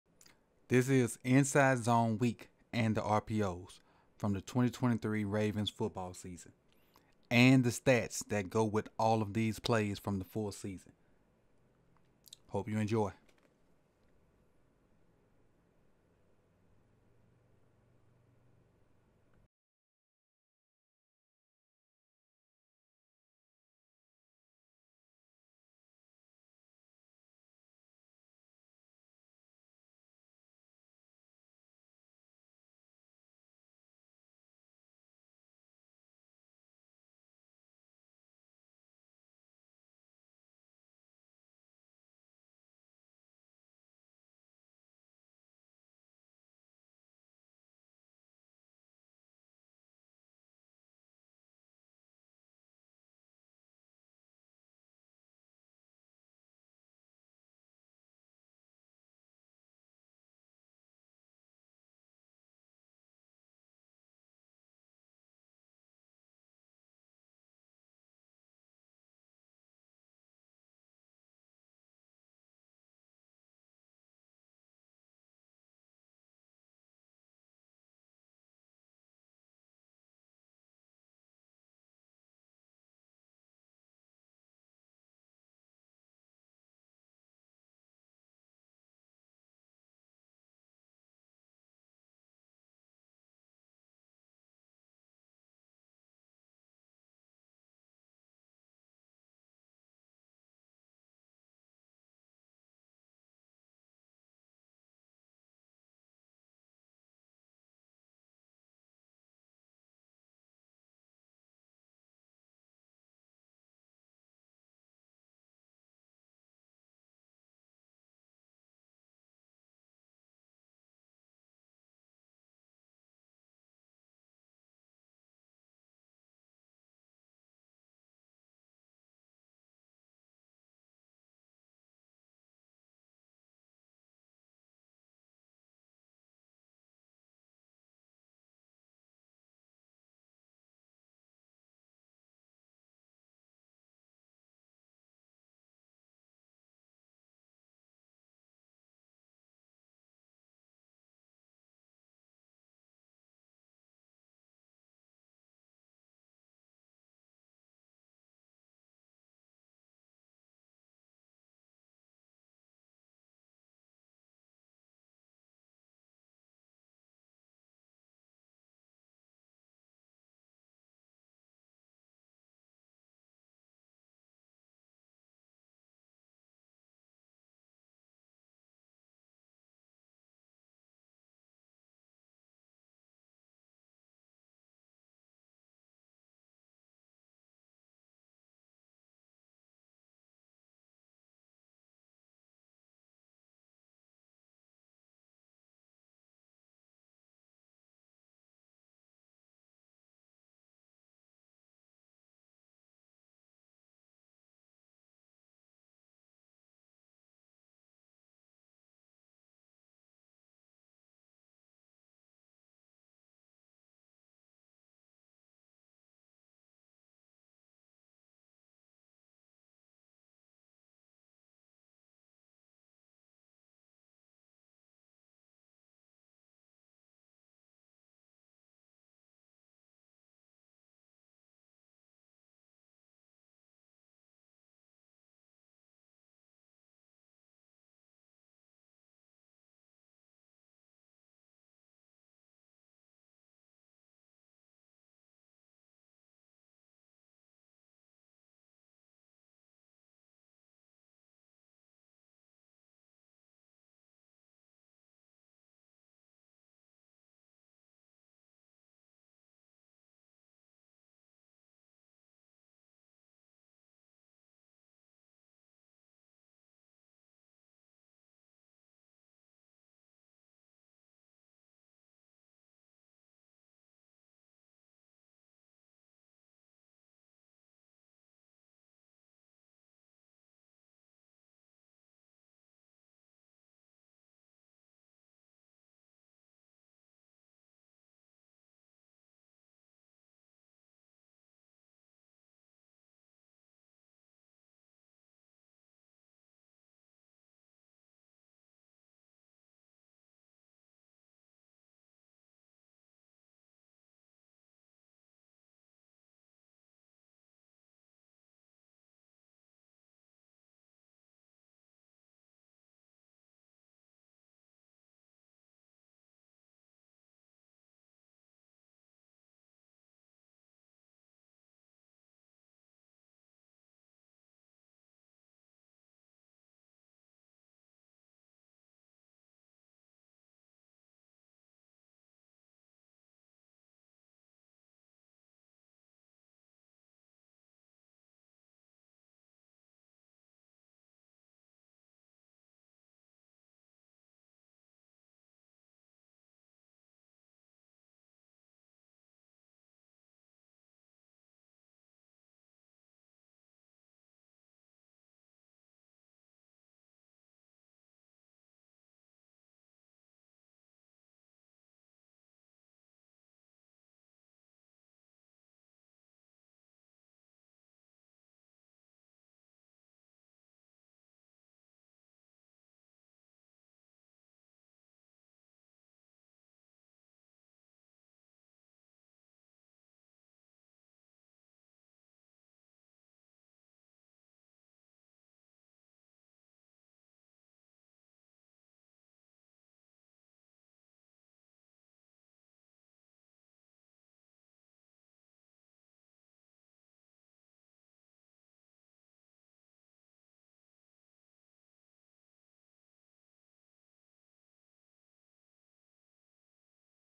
2023 Ravens Inside Zone Weak & Rpo's
From the 2023 Season Inside Zone weak and the Rpo's with it.

*** Send Stuff***
2241 North Monroe St 1267